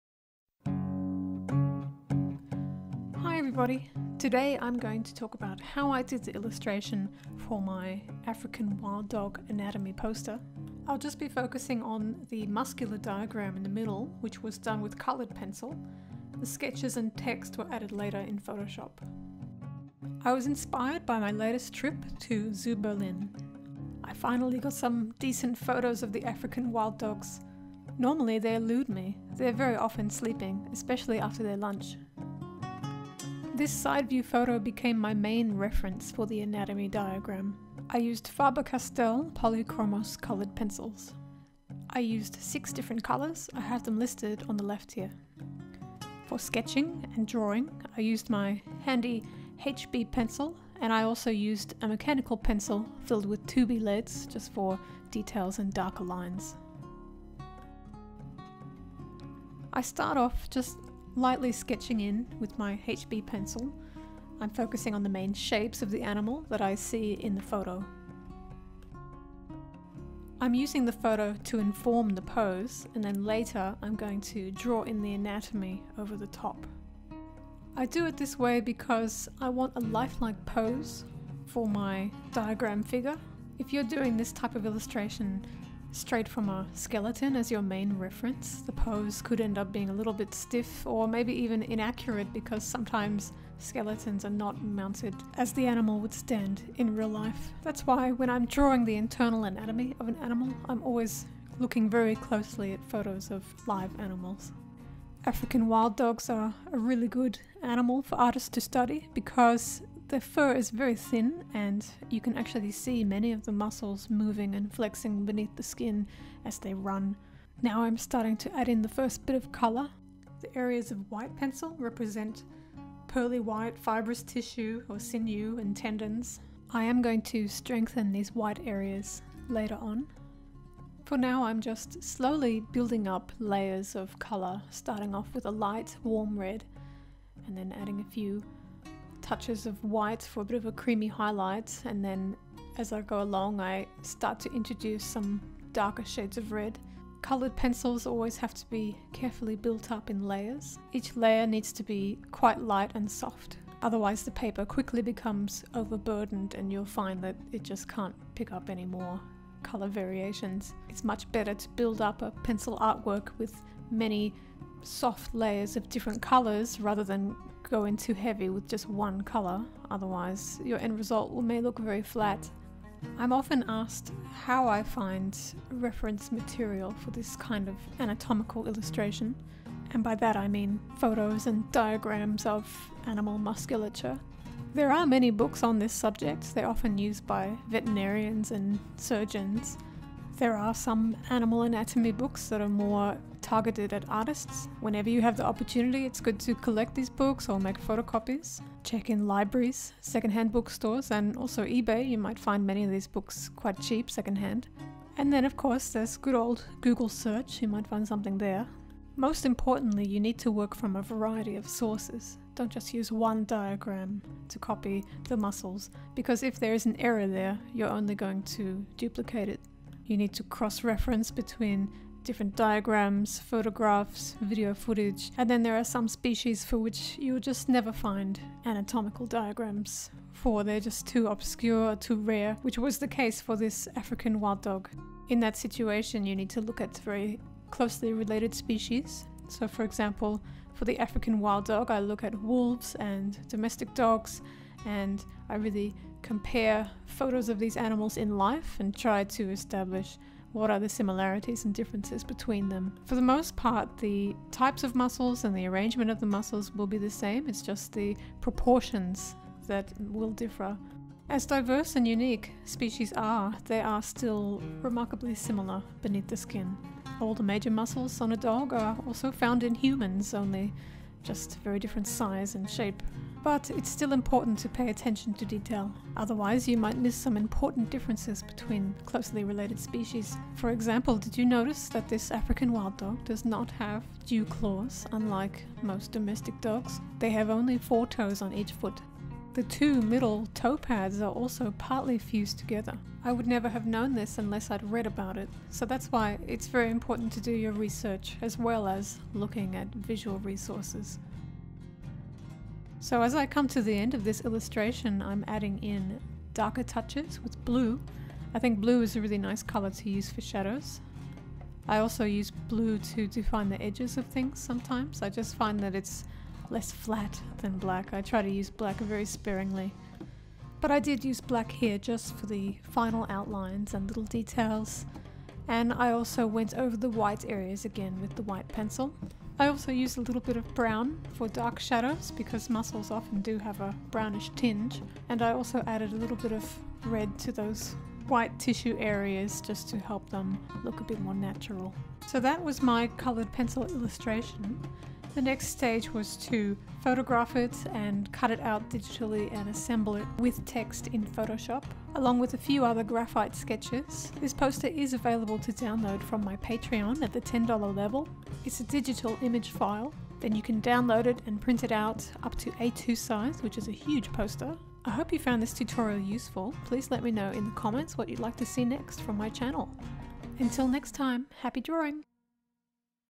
Pencil Drawing Tutorial | African Wild Dog Anatomy
Today I talk about my process researching and illustrating this African wild dog anatomy diagram.

➤ Art supplies used:
HB + 2B graphite pencils
Faber-Castell Polychromos pencils
Strathmore toned paper

Animal art and tutorials by Pip Abraham.